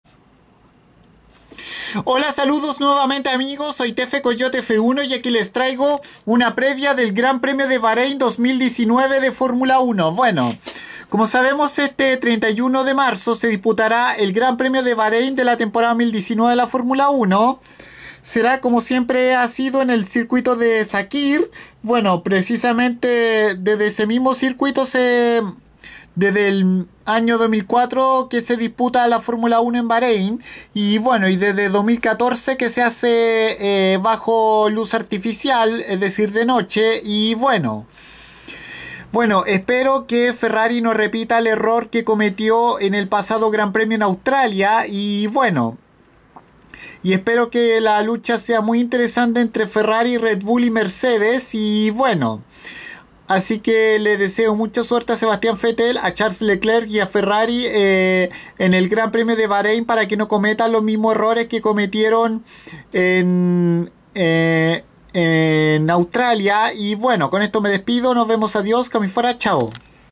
Previa GP de Bahrein 2019 de F1
Una breve previa del 2° Gran Premio de la Temporada 2019 de la F1 que se correrá en la pista árabe de Sakhir en Bahrein